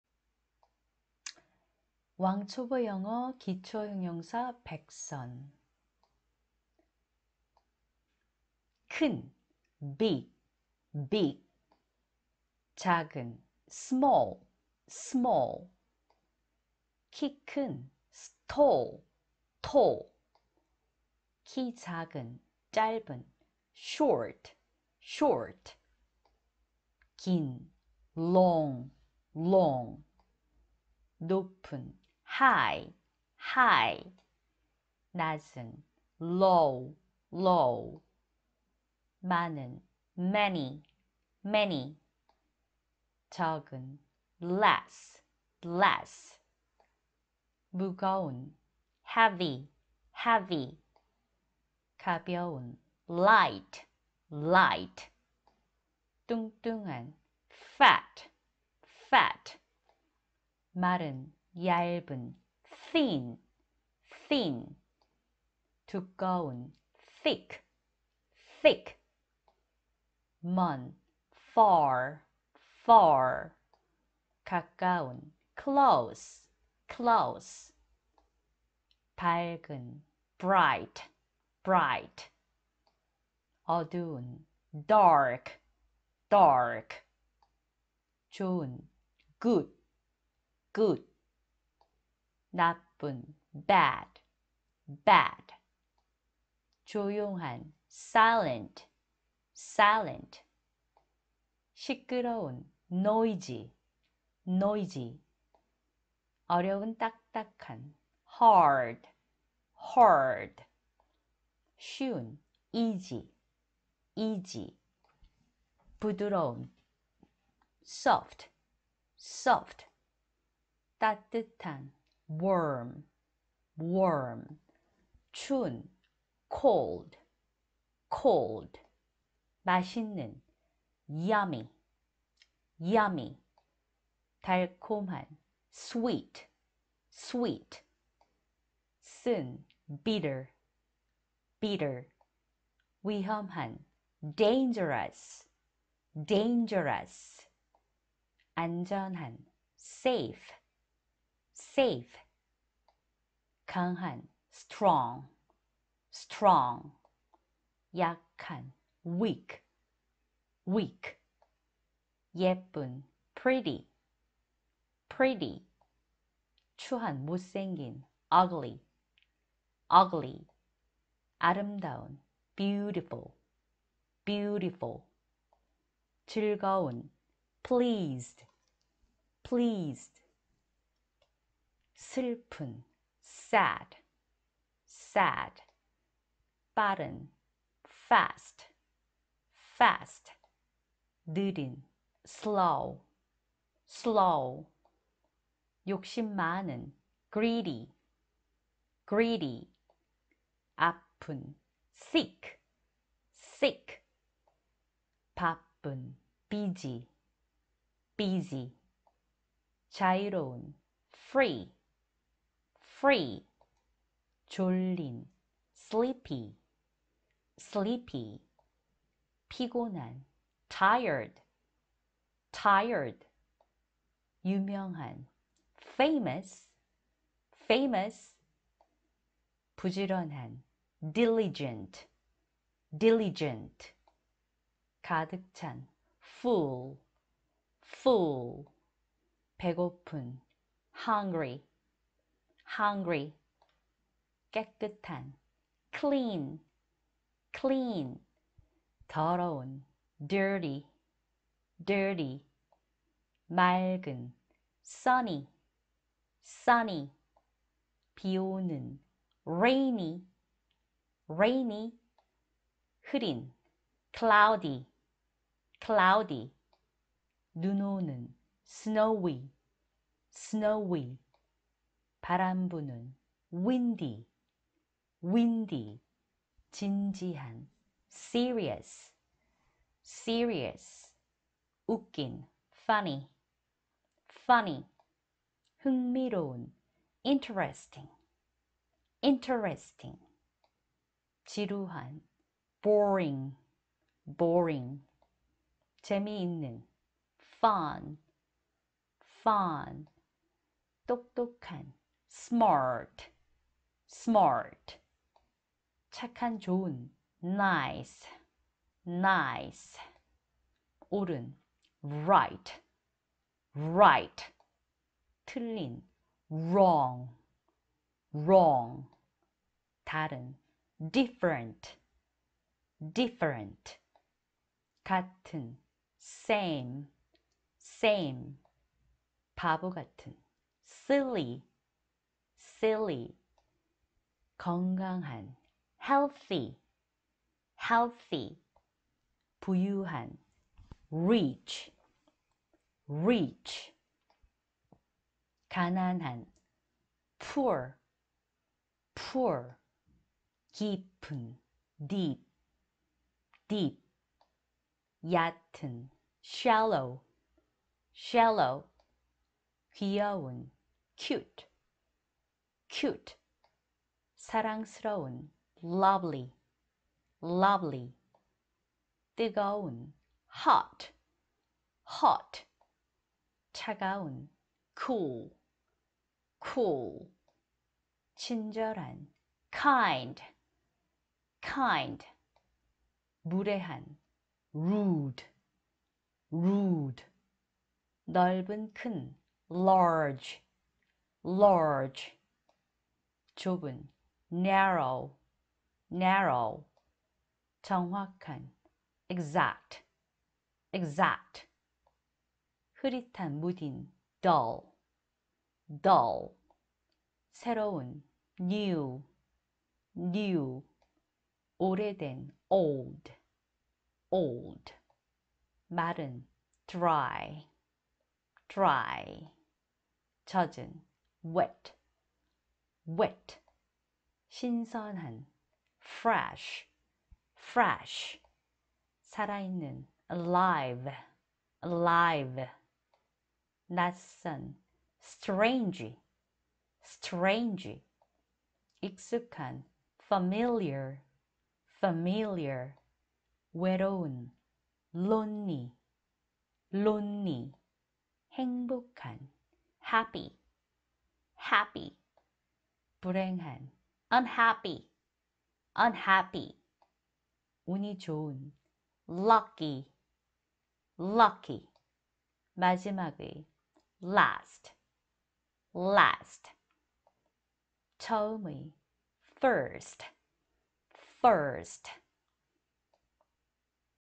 생활영어단어연속듣기 기초형용사100선(2) study Korean - adjective 100 words
왕초보 영어

. 기초 형용사
studyKorean - adjective 100 words

큰소리로 따라서 말하고 스스로 발음 연습하세요.